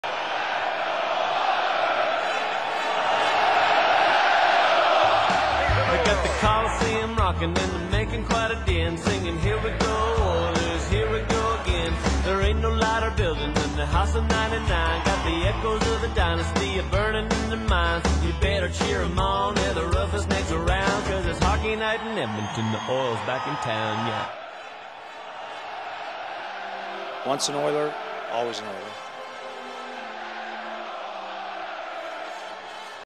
Oilers Animation
A kinetic typography animation I made mixing my love of hockey with my film studies.

The Oilers are my favourite team and as I started this I felt very impacted by the Oiler community response to Dave Semenko's passing so I tried to make it a small tribute as well.